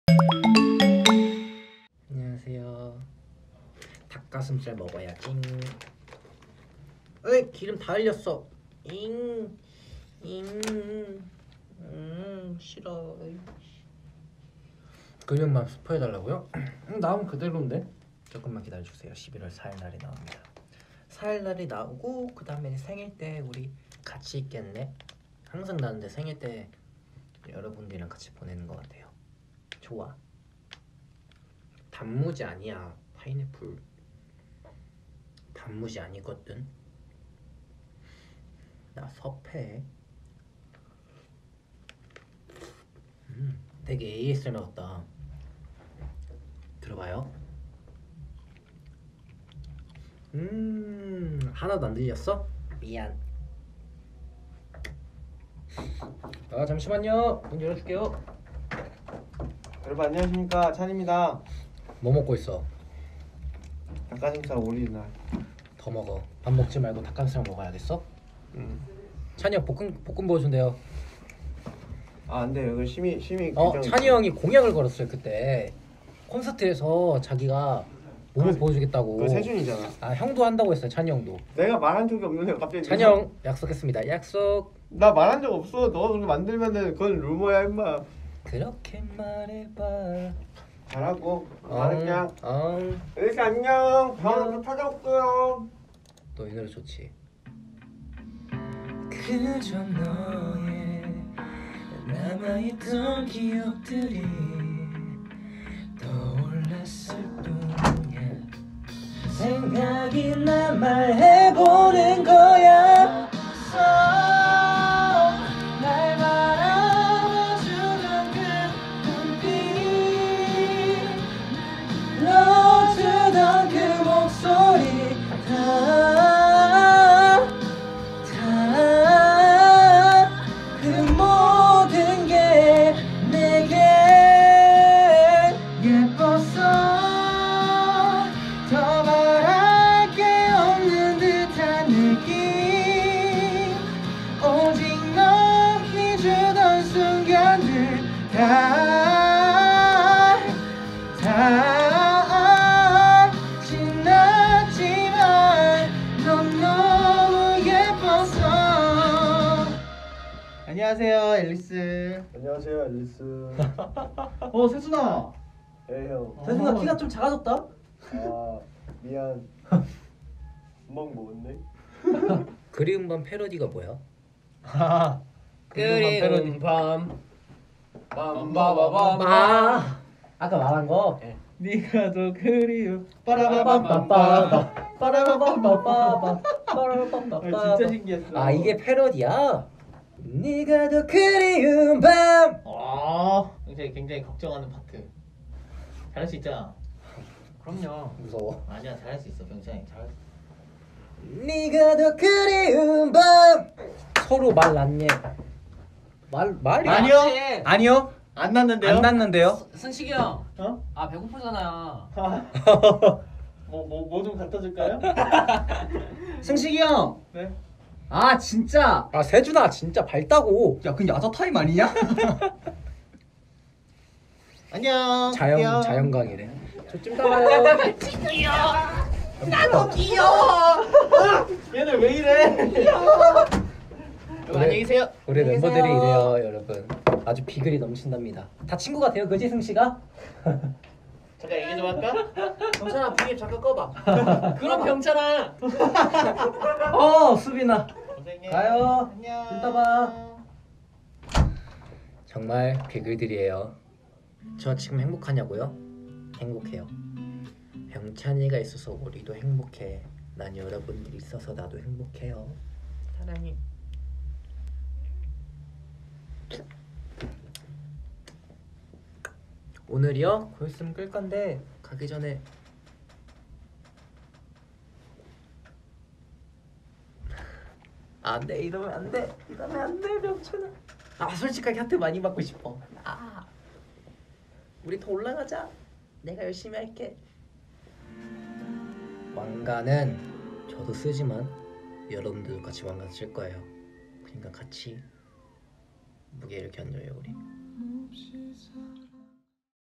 [V LIVE] VICTON - 형아 존중 대체 웨얼? 이것은 말 놓기인가 야자타임인가💥 (VICTON talking about their new song)
★ [Full] VICTON - 월요천사 병차니!!💕🐥🐶

★ Enjoy K-POP celeb's LIVE broadcast with English subtitle on V LIVE!
​​더 많은 셀럽의 영상을 보려면? V LIVE 다운로드!